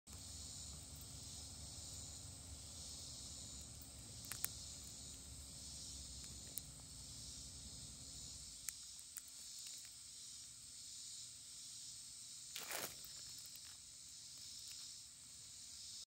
Snail and Slug Eating Mushroom | Hiking Adventures
Out hiking on some of our usual trails, we saw both, a snail and slug eating this almost devoured mushroom. If you look closely, you'll also see a cute little spider who made an appearance as well.

Land Snail (Mesodon thyroidus)

Mesodon thyroidus is a species of air-breathing land snail, a terrestrial gastropod mollusk in the family Polygyridae. (Wikipedia)

Land Slug (Philomycus carolinianus)

Philomycus carolinianus, common name the Carolina mantleslug, is a species of land slug, a terrestrial gastropod mollusk in the family Philomycidae. (Wikipedia)